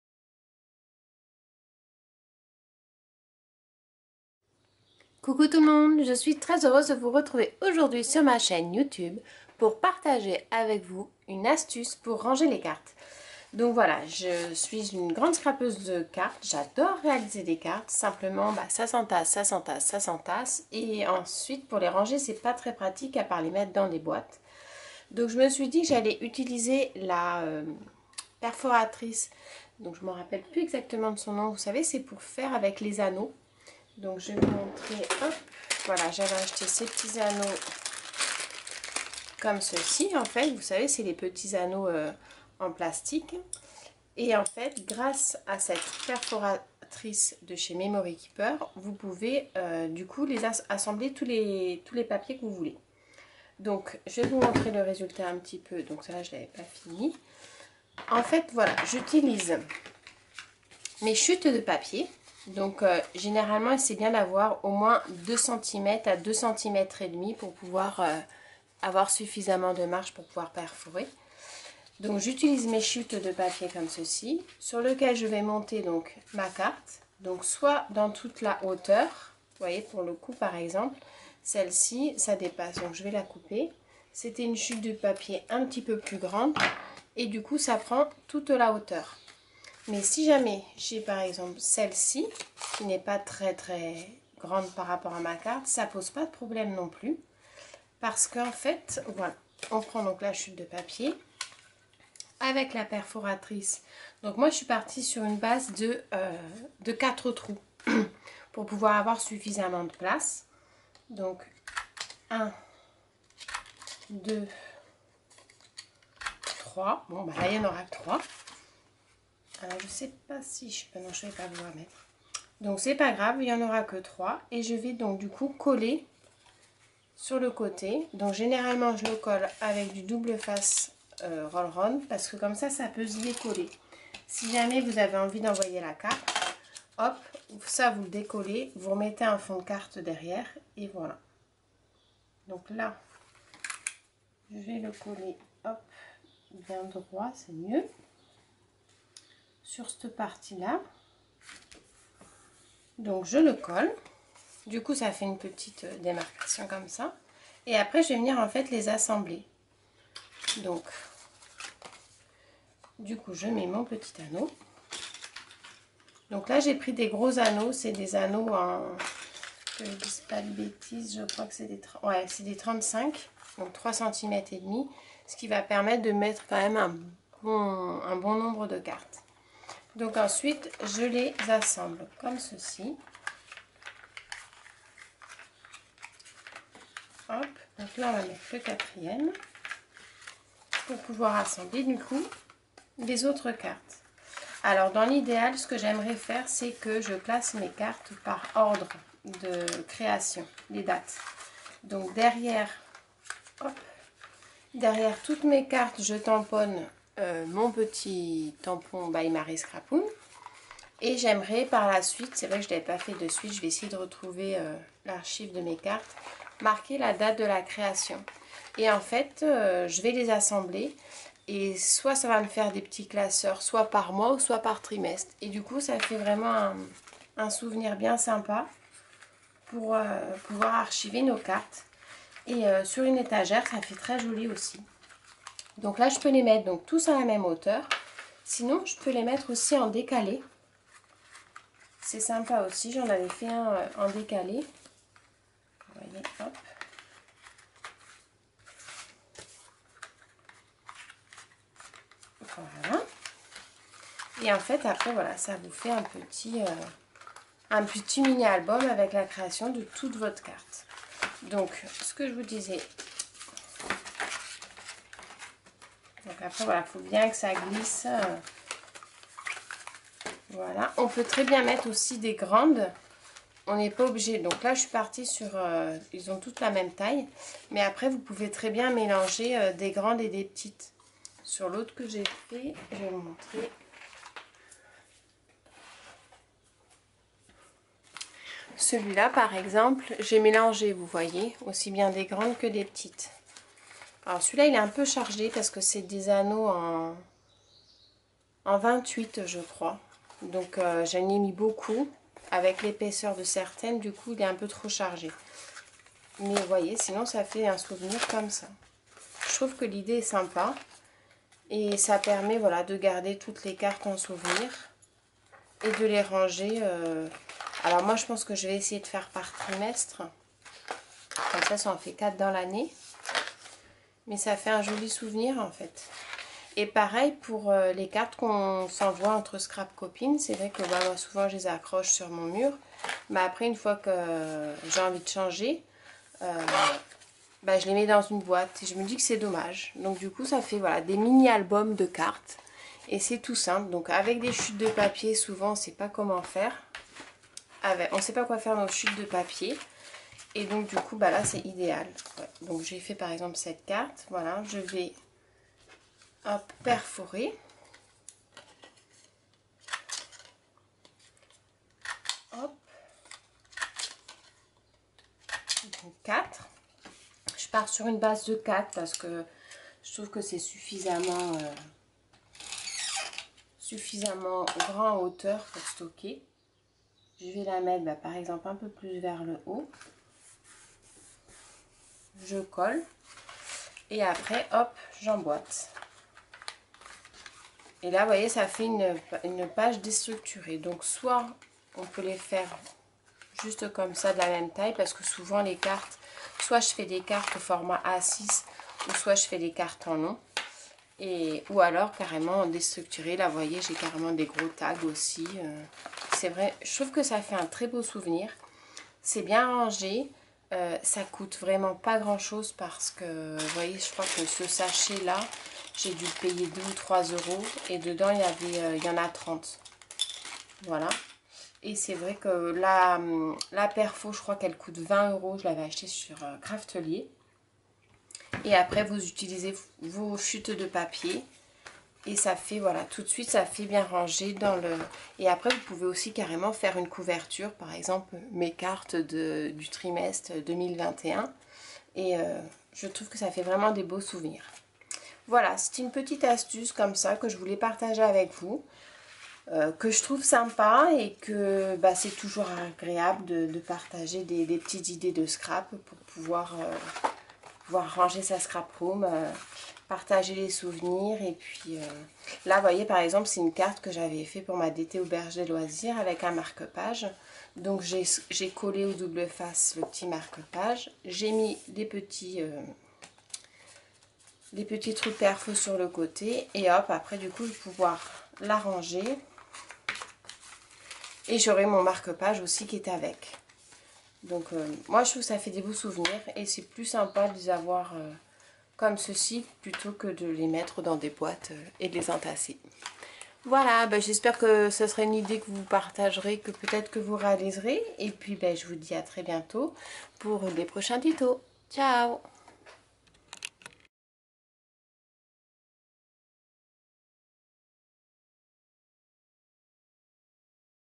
ASTUCE RANGEMENT Carterie Scrapbooking, TAG, Facile, rapide, jolie et pas cher. By Marie Scrapoune.
Je partage avec vous une petite astuce pour ranger vos cartes joliment et surtout à moindre coût :)
Très belle journée à tous et à très bientôt.

Marie Scrapoune